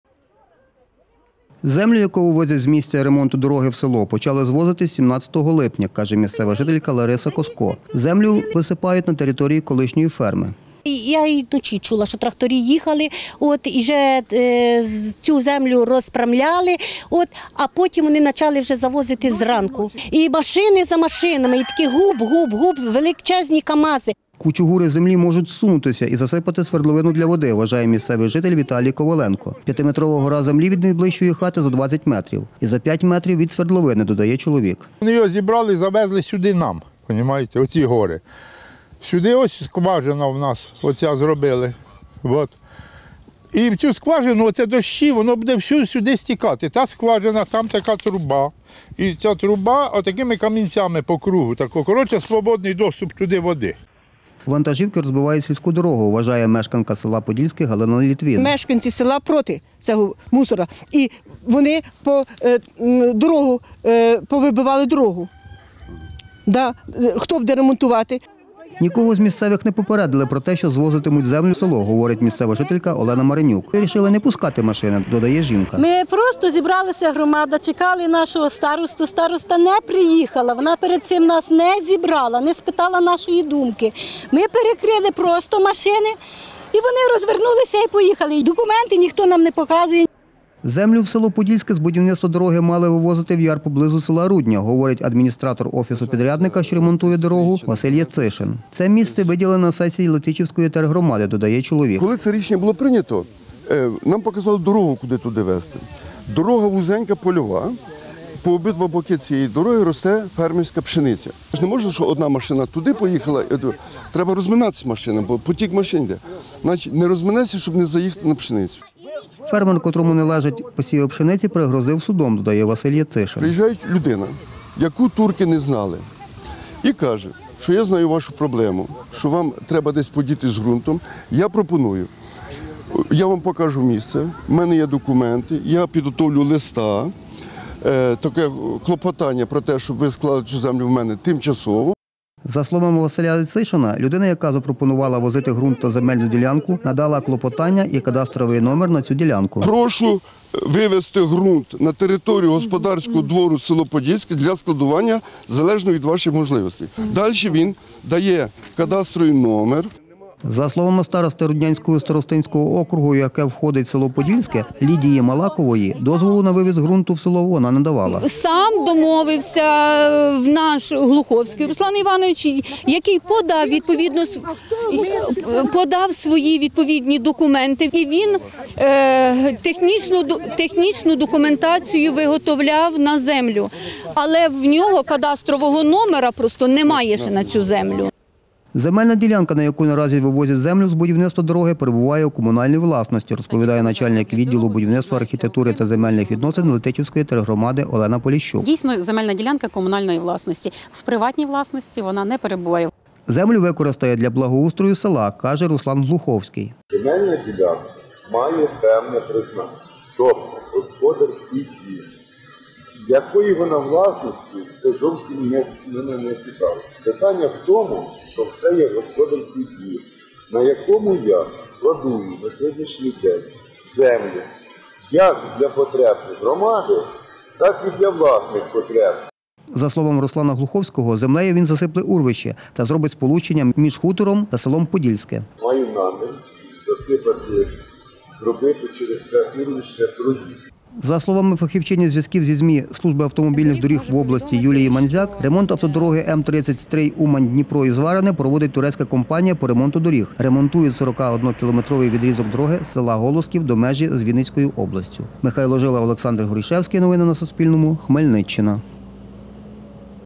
На Хмельниччині грунт з ремонту магістральної траси вивозять у село. Місцеві протестують
Жителі села Подільське Хмельницького району протестують проти того, аби до них звозили грунт, що збирається під час будівництва та ремонту дороги у напрямку Летичева. Кажуть, бояться, що земляний вал, який знаходиться близько хат та свердловини, може зсунутися.
Про це Суспільному розповів місцевий мешканець Віталій Коваленко. Чоловік каже, люди вирішили не пускати вантажівки з землею до села.